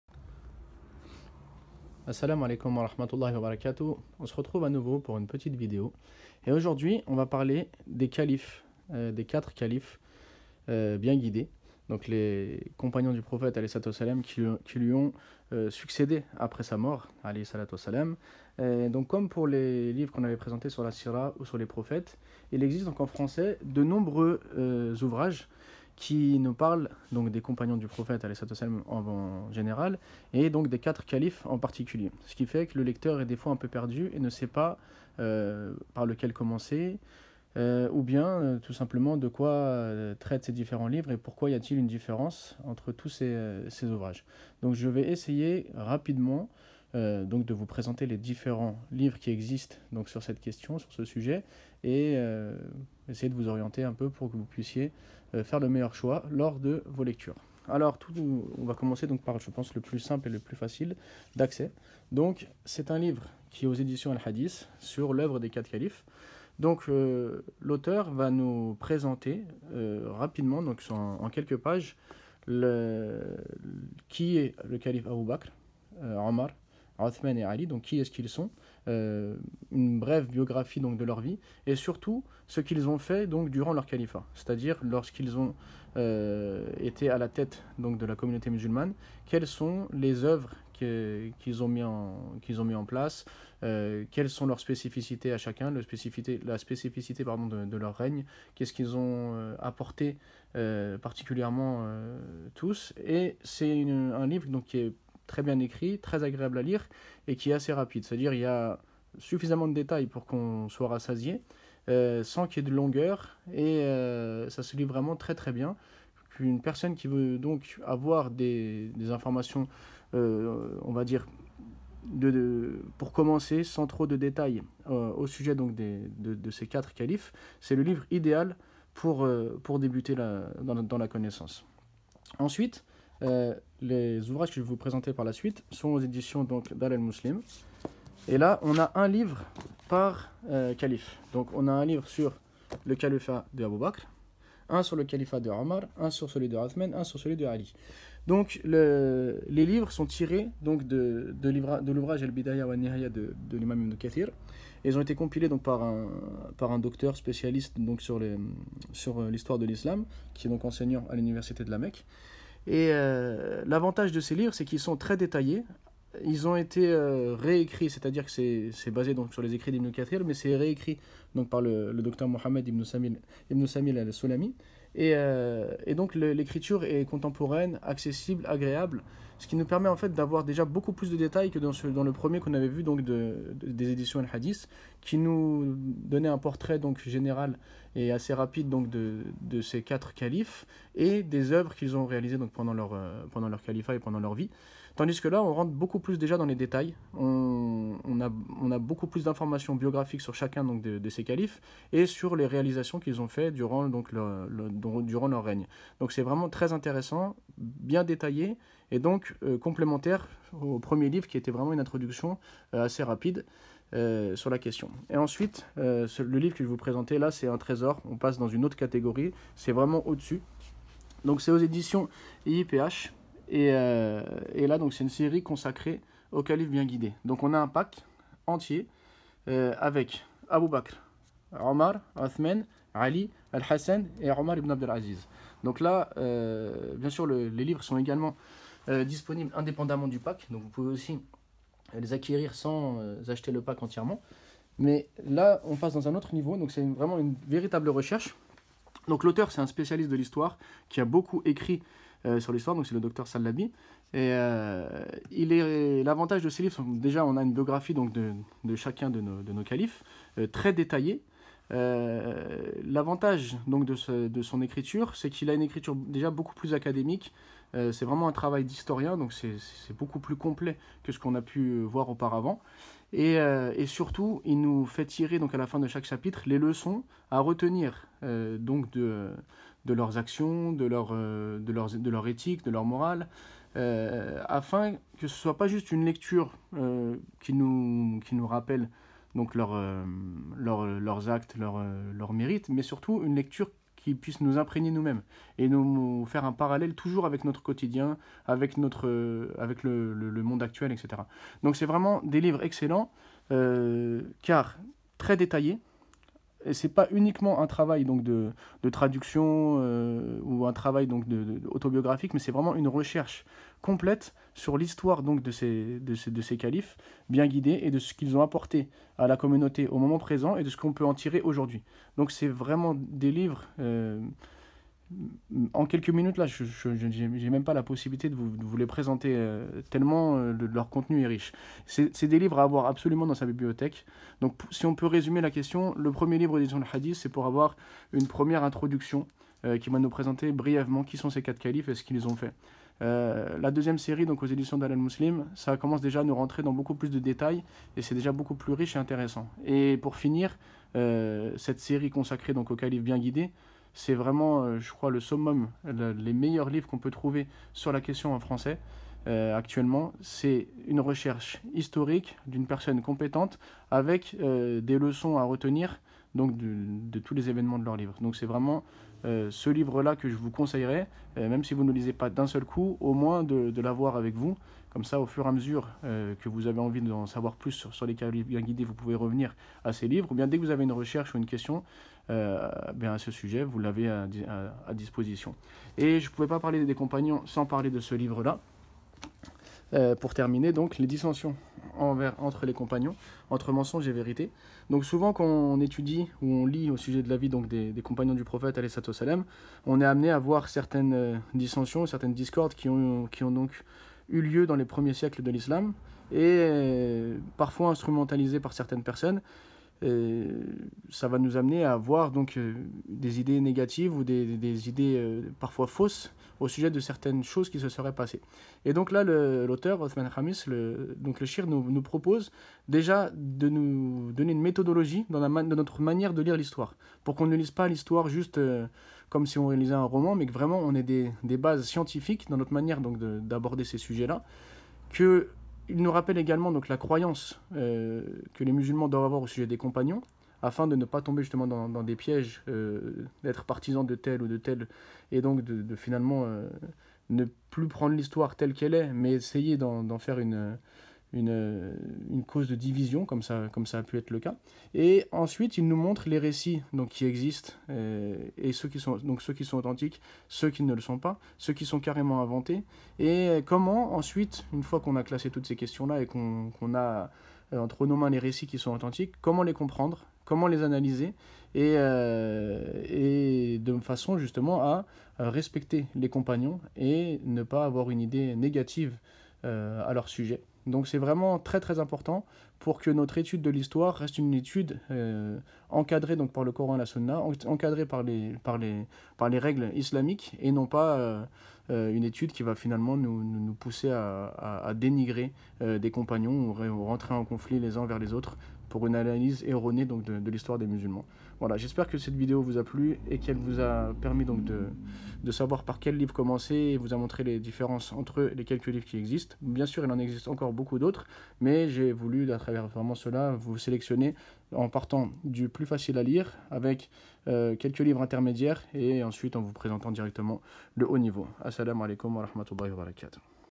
Les 4 califes
Il existe de nombreux livres traitant des califes ayant succédés au prophète ﷺ .
Cette vidéo vous propose dé découvrir la différence entre ces différents livres afin de faire le meilleur choix.